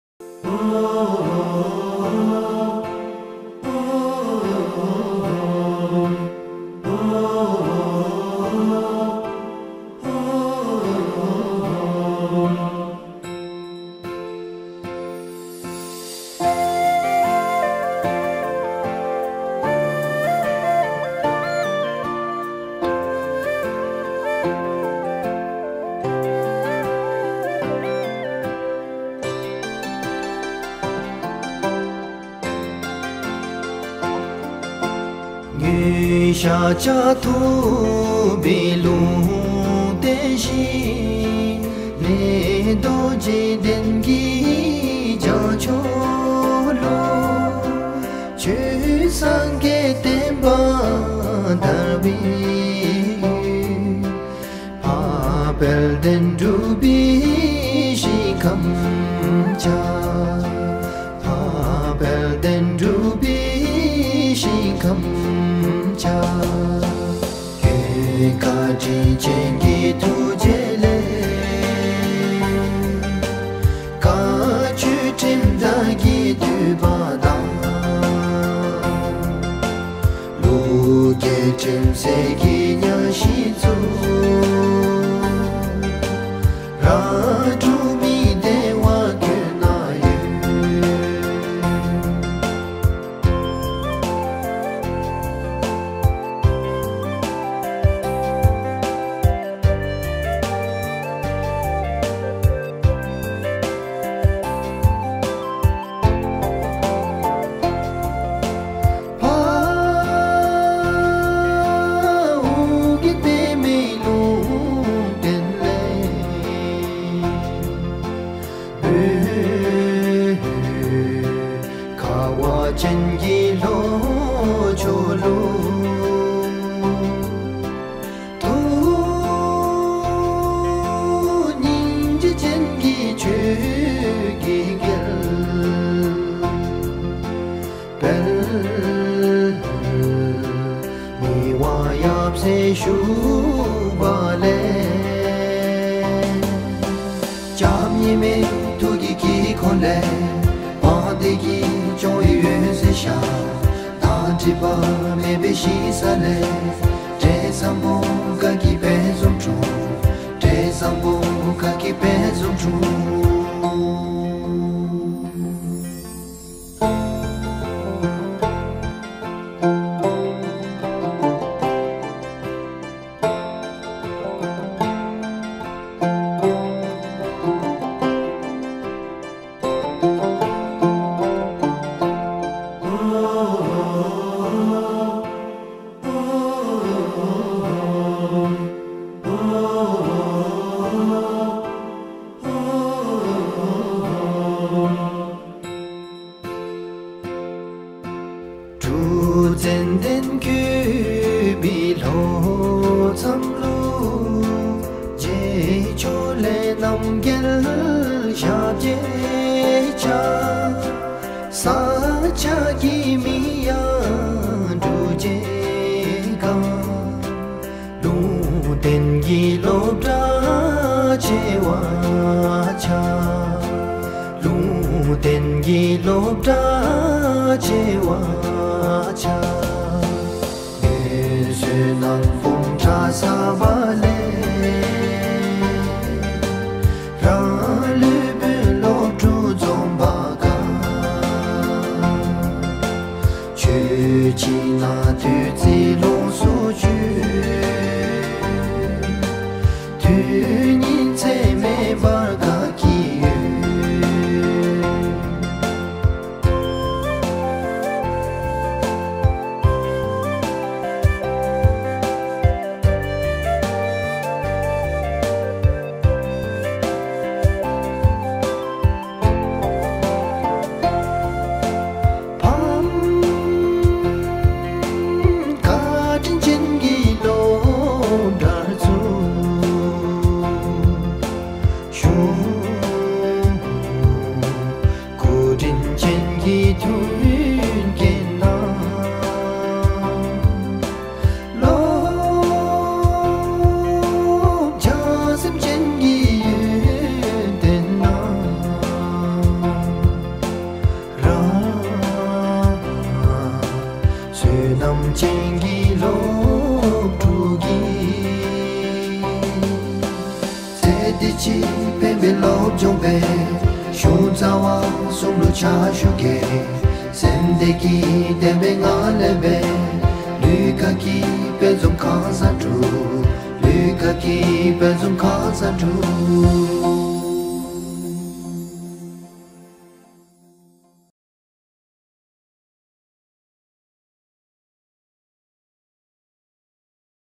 Drukjegang HSS (Bhutan) Music Video
A music video for Drukjegang Higher Seconday School highlighting some of the school activities and events throughout 2015. Music written by Lopen Sangay Dorji.